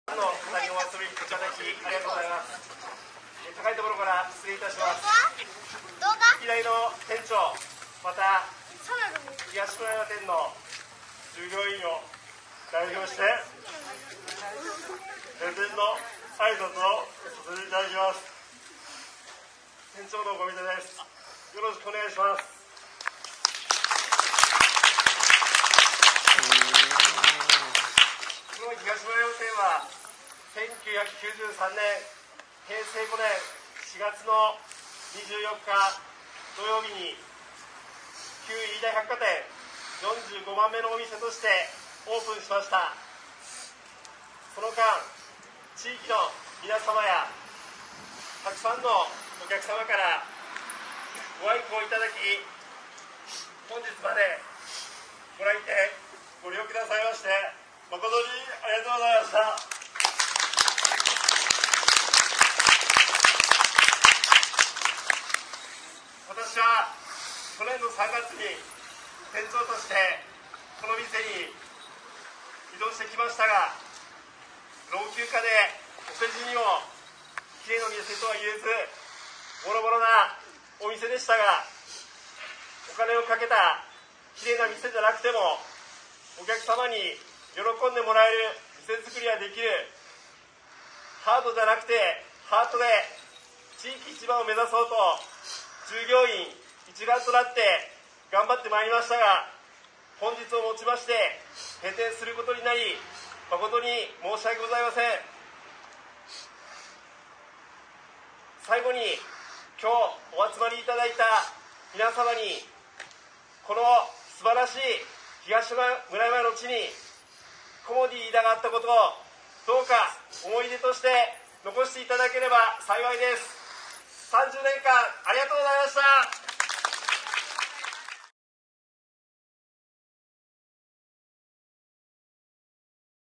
30年の歴史に幕下す コモディイイダ東村山店閉店
2023年3月15日
約30年間営業して参りましたコモディイイダ東村山店が閉店することとなりました。
何とか継続できる道を探し、全社で力を入れてまいりましたが、地域の皆様には本当に申し訳ございません。
また永年に亘り東村山店で勤務をしてくださったパートナーの皆さん、アルバイトの皆さん、本当にありがとうございました。